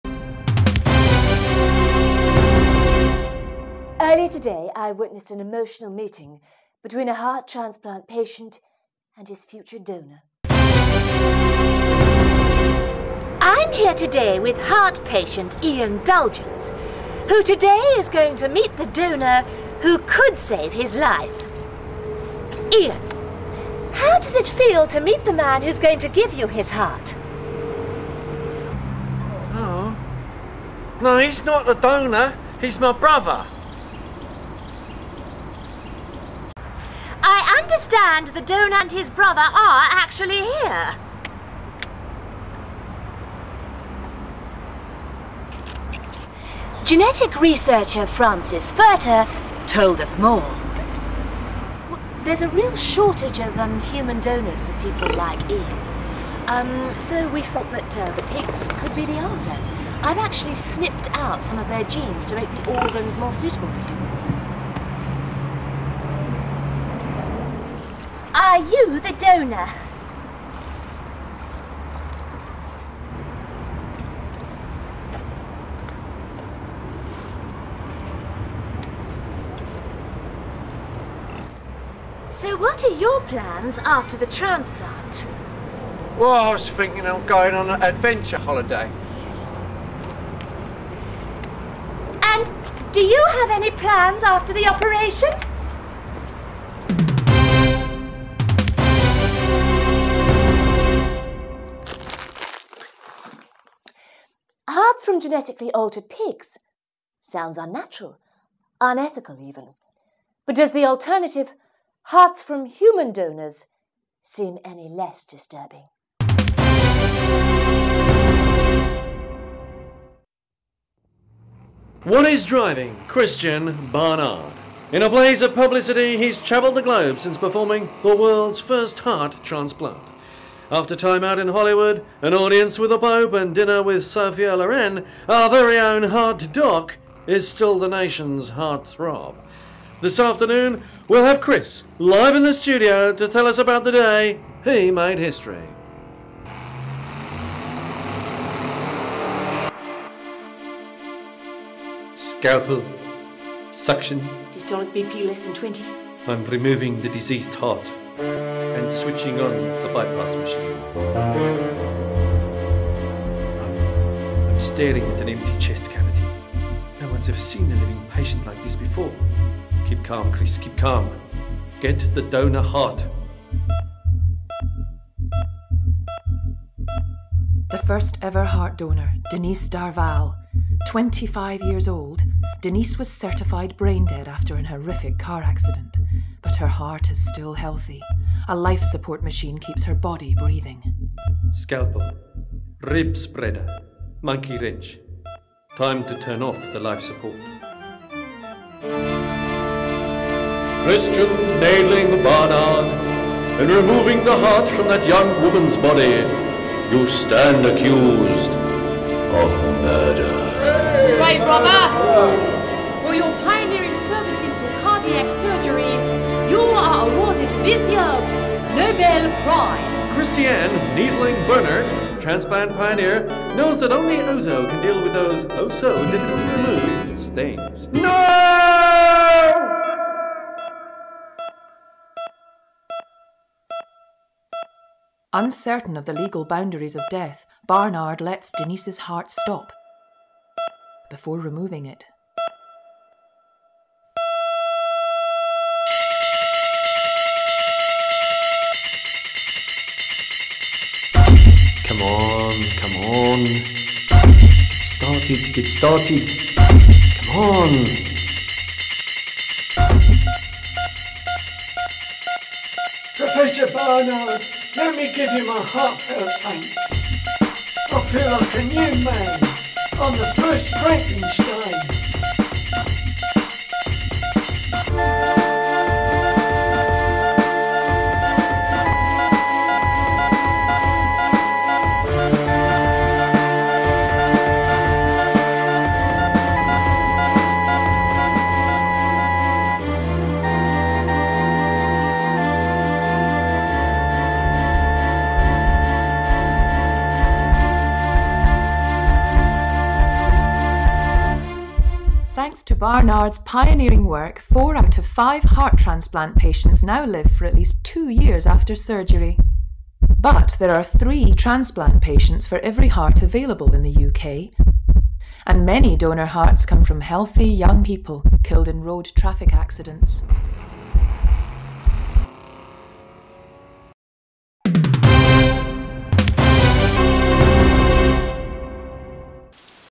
Christian Barnard and his hearts
The story of the surgeon Christian Barnard, told with animatronics. This is part of an object theatre in Glasgow's science centre. It was made by Tim Hunkin, Sarah Angliss and will Jackson